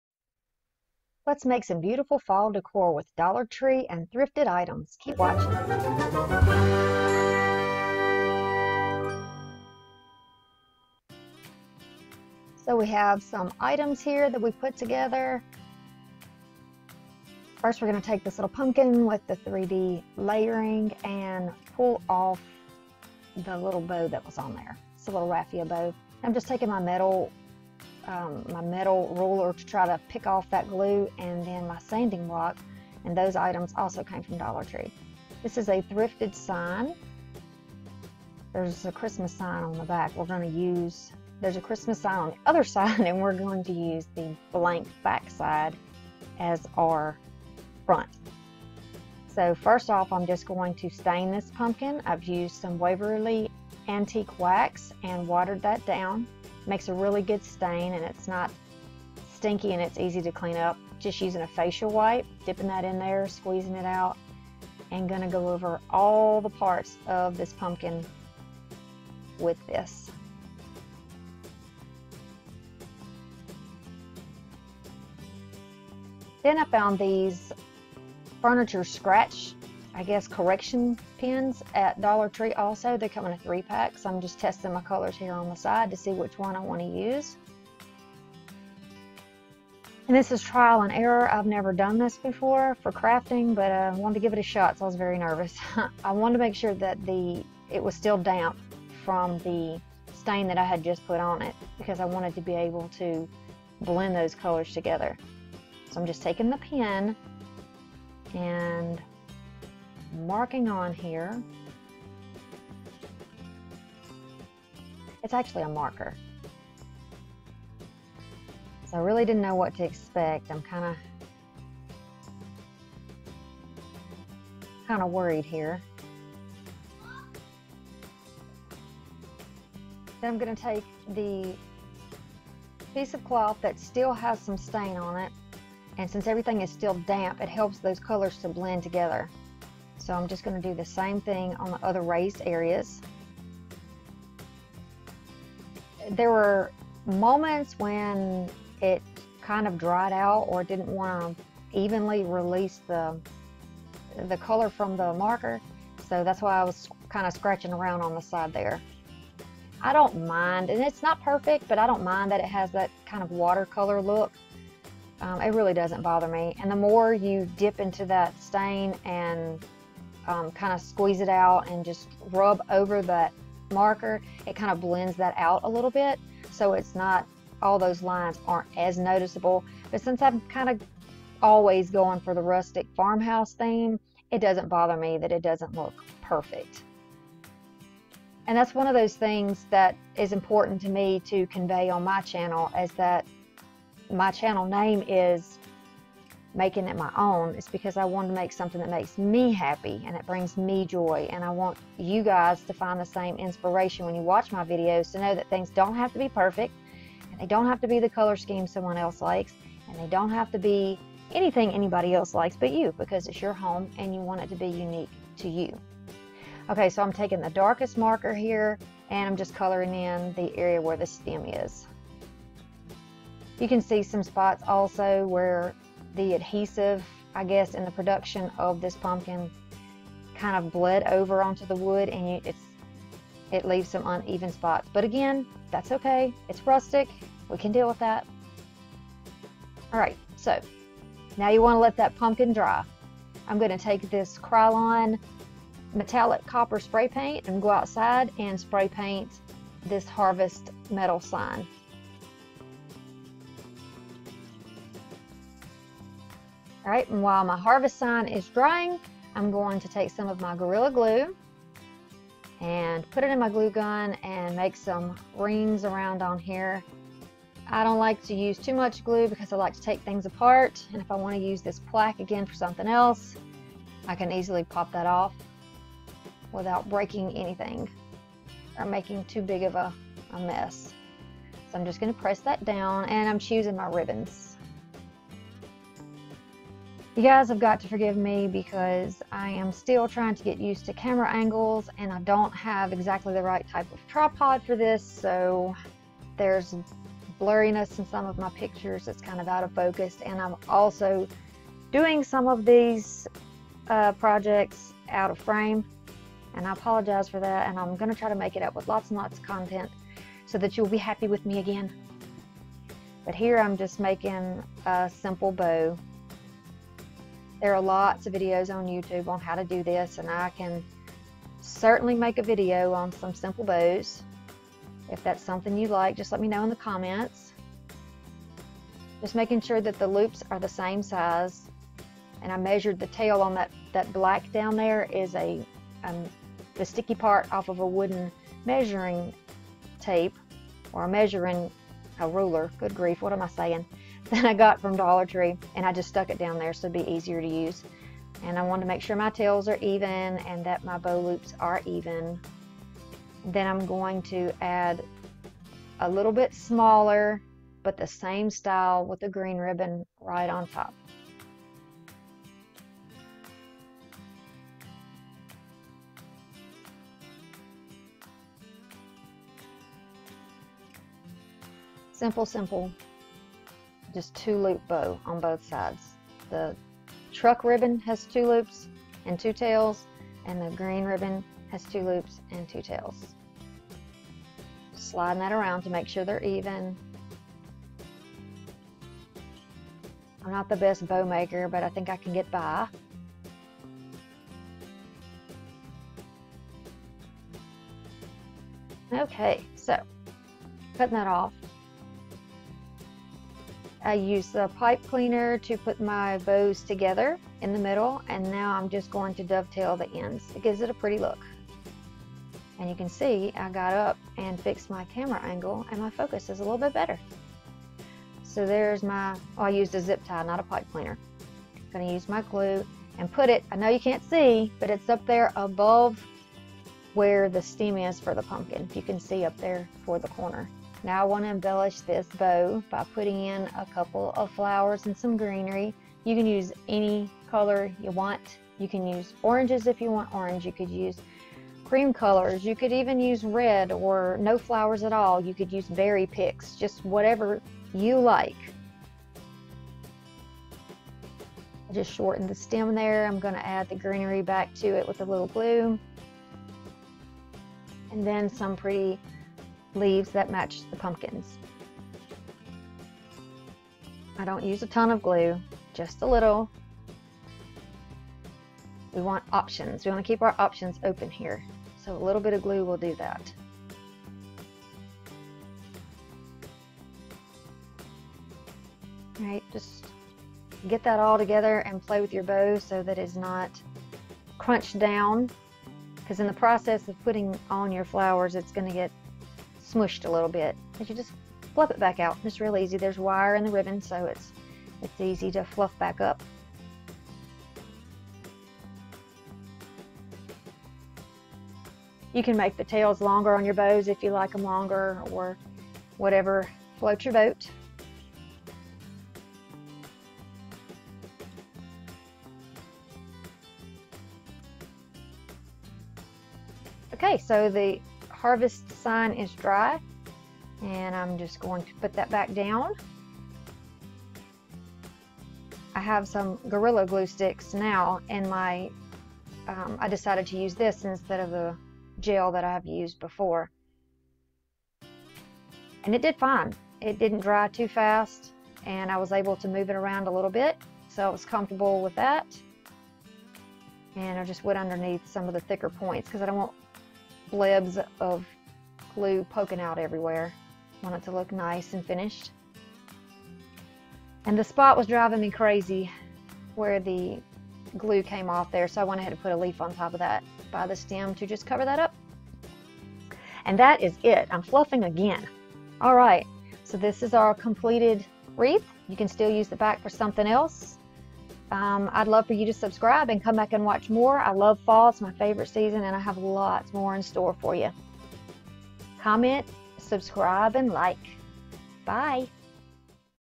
Dollar Tree Wooden Pumpkin Sign Copper Accents - Fall 2020
In this video, I will show you how to DIY this rustic fall sign or hanging decor with Dollar tree items! The plaque is from either goodwill or dirt cheap, I've had it a while in my stash. Supplies you need include (but are not limited to) Glue sticks and glue gun, a few wired ribbons of your choice, a wooden pumpkin, floral and greenery, floral wire, metal sign pack of words, Krylon metallic copper paint, furniture scratch markers, waverly antique wax paint, wipes, water, small bowl to mix your paint. You ready?